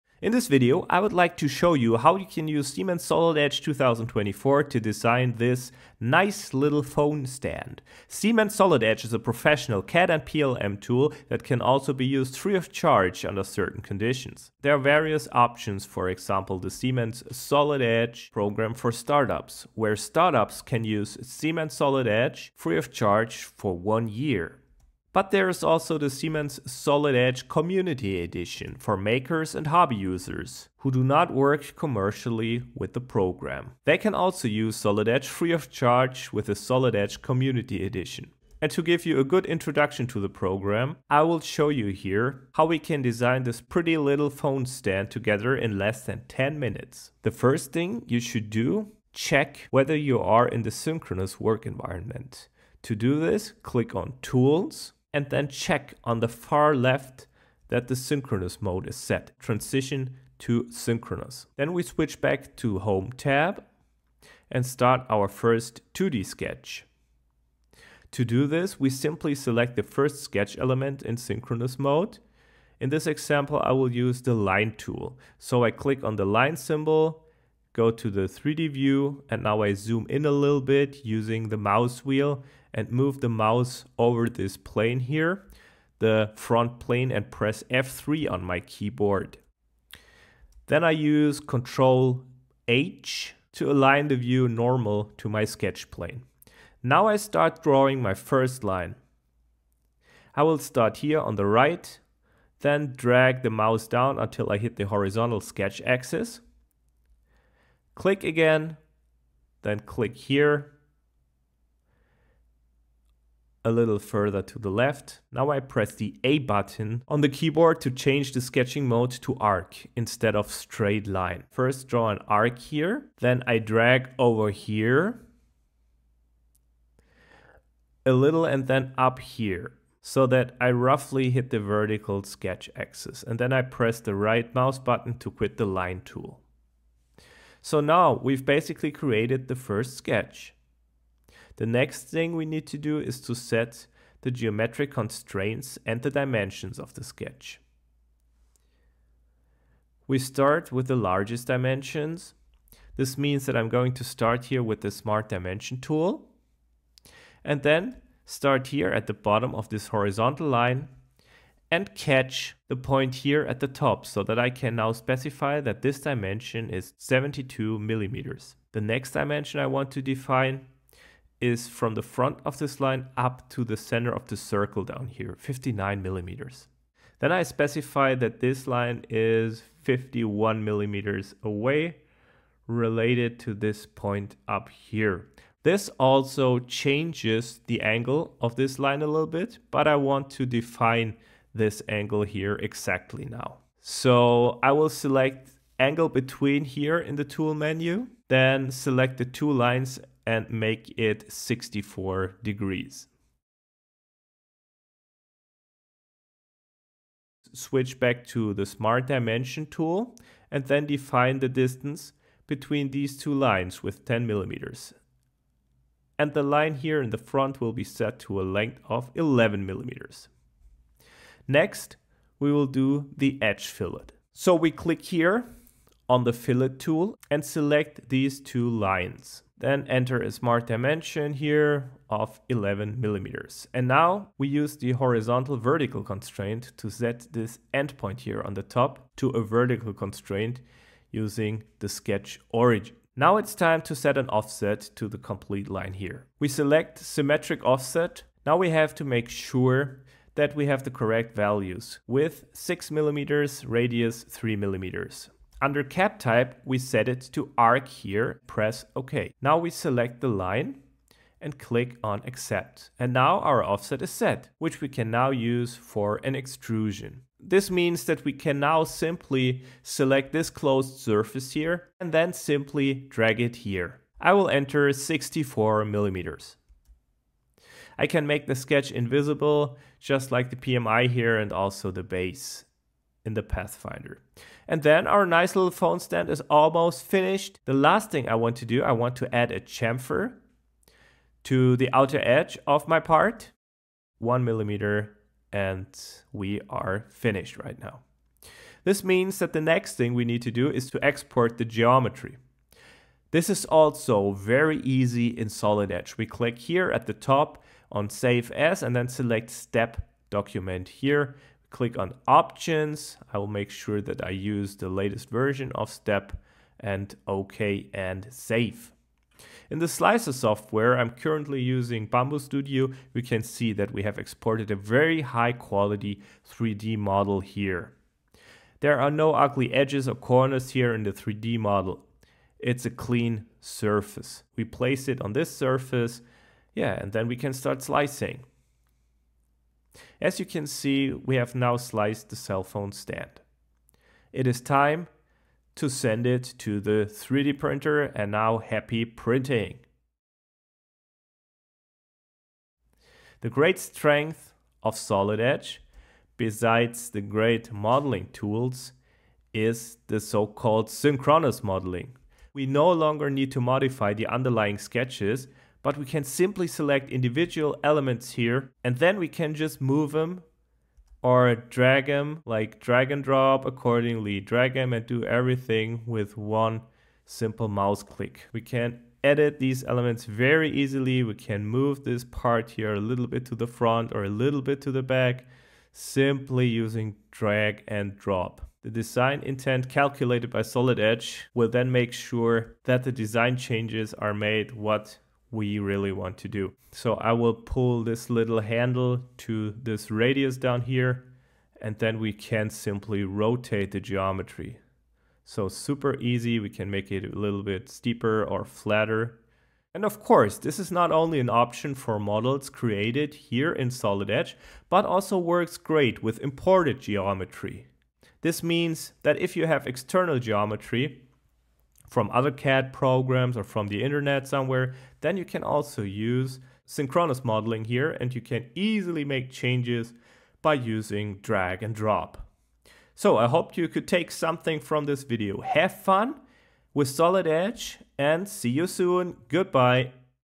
Your first model for 3D printing in Siemens Solid Edge for free.
Unlock the power of 3D printing with this quick and beginner-friendly tutorial!
In under 5 minutes, you'll learn how to design your own smartphone stand using the free version of Siemens Solid Edge 2024 with its cutting-edge Synchronous Modeling. Perfect for anyone eager to dive into 3D design and rapid prototyping. Watch now and start creating your own 3D models today!

Chapters: 📓
00:00 Introduction
00:08 Get Solid Edge Free
00:40 Start Design
00:48 Enable Synchronous
1:23 Select Plane
1:37 Draw Sketch
2:40 Add Dimensions
04:03 Offset Sketch
4:27 Symmetric Extrude
04:48 Add Chamfer
05:01 Export STEP File
05:20 Check in Slicer
05:50 Print Model
05:56 Synchronous Tips
07:13 Outro